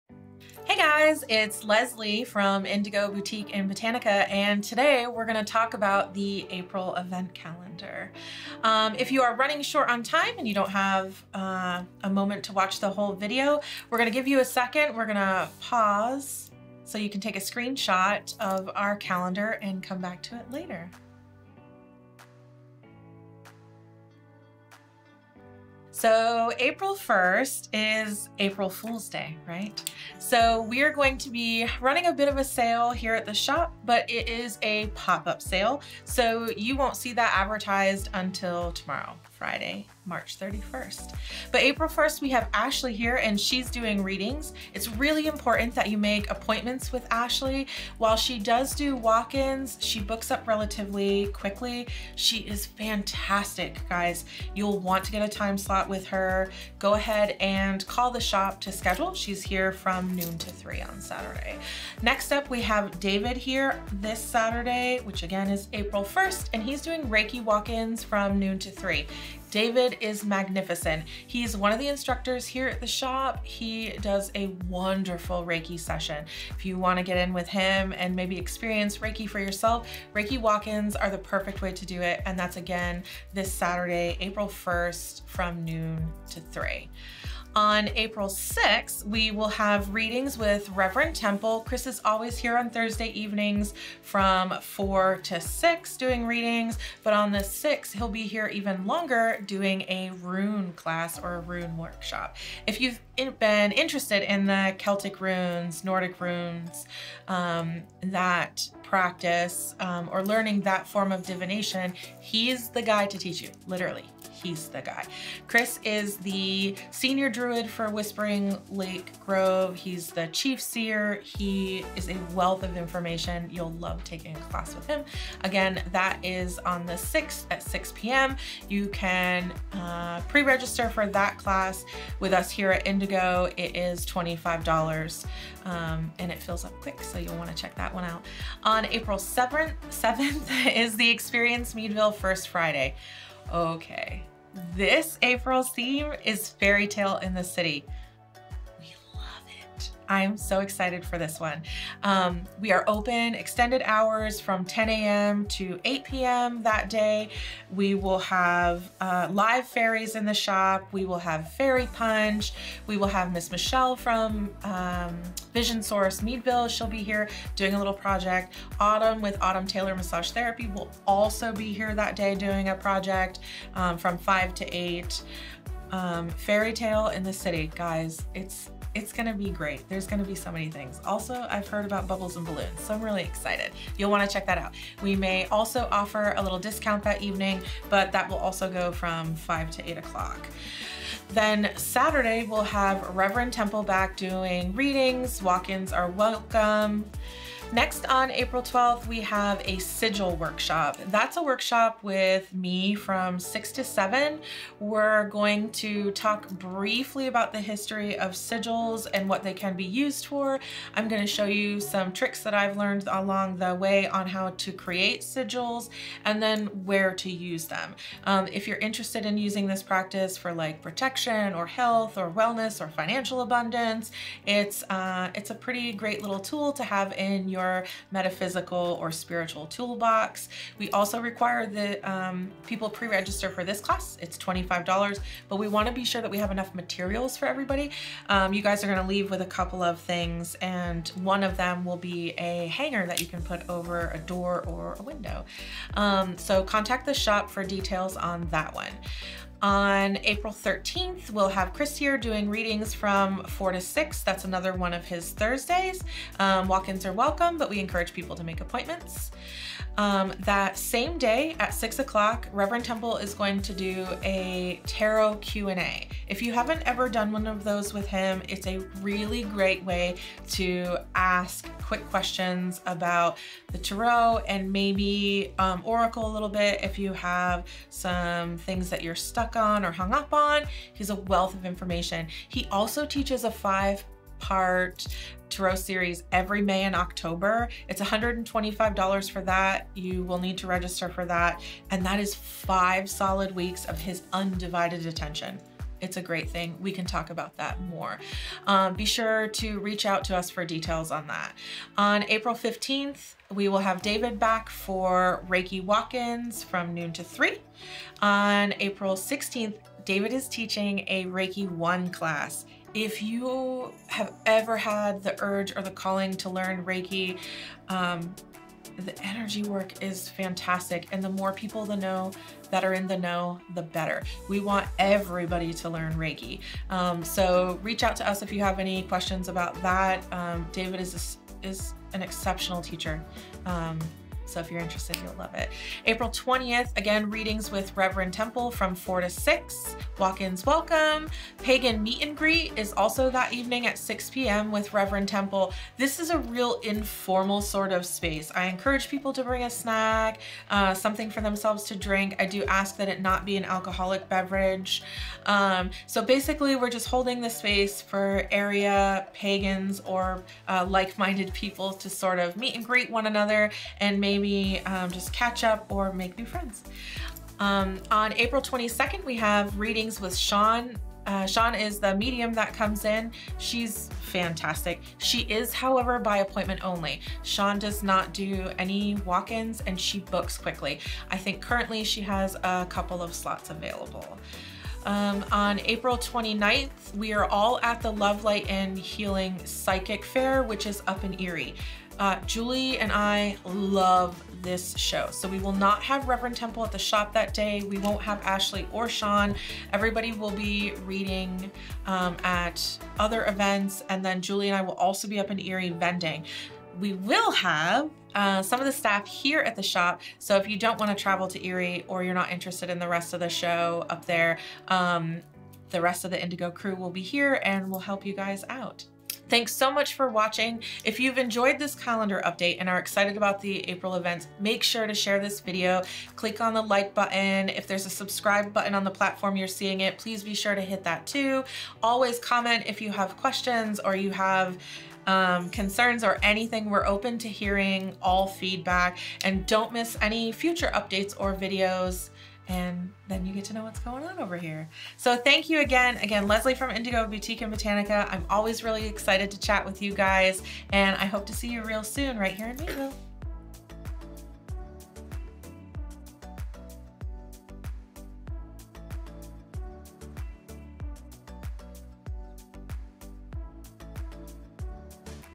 April Events Calendar Update: April 2023
Hey Friends!  Here’s some of what’s happening at Indigo and in the downtown for the month of April. If you are short on time pause for a screen shot is available at the beginning of the video. For all the details, stick around and Leslie will tell you all about it!